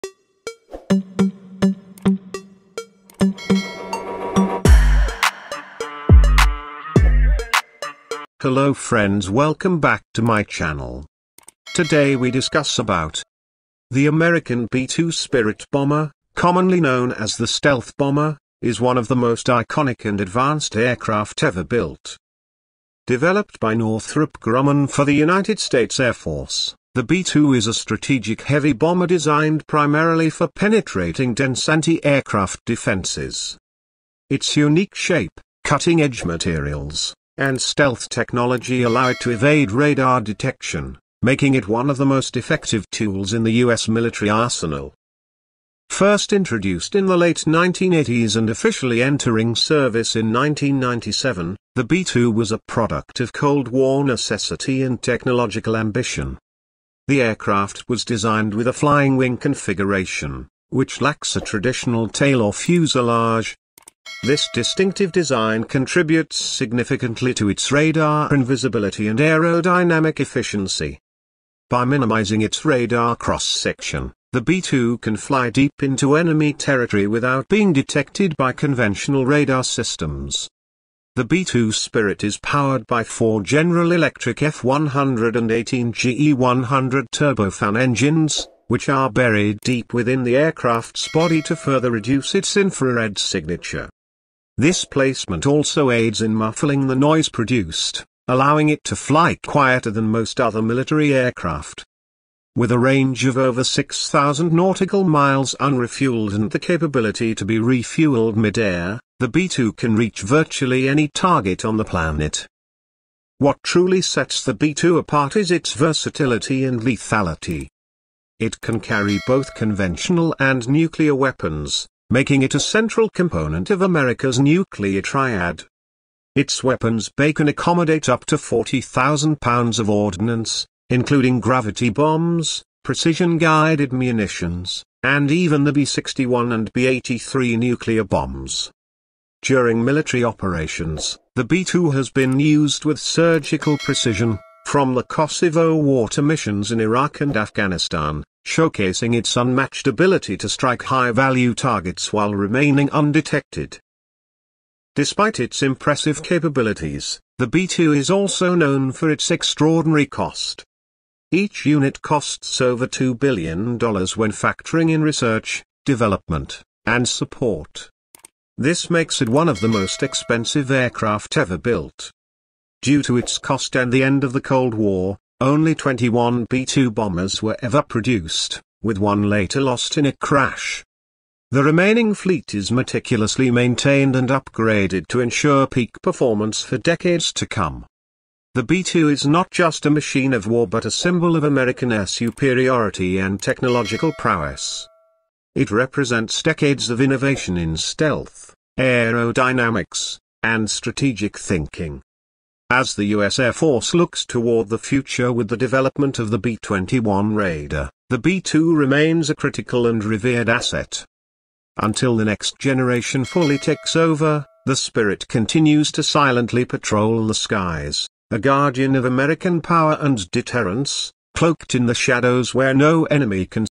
Inside the B-2 Spirit: America’s Secret Flying Wing
Step into the world of cutting-edge stealth technology with an in-depth look at the American B-2 Spirit bomber—one of the most advanced and mysterious aircraft ever built. Known for its sleek flying-wing design and radar-evading abilities, the B-2 Spirit remains an unmatched symbol of American air dominance. This video explores the history, design, technology, and missions of this $2 billion marvel, explaining why it still rules the skies decades after its first flight.

From its role in high-risk missions to its place in the U.S. nuclear triad, the B-2 is more than just a bomber—it's a ghost in the sky. Learn how it operates undetected, what makes it so unique, and why no other country has matched its capabilities. Whether you’re an aviation enthusiast, military tech fan, or just curious about stealth aircraft, this video dives deep into everything you need to know about the B-2 Spirit.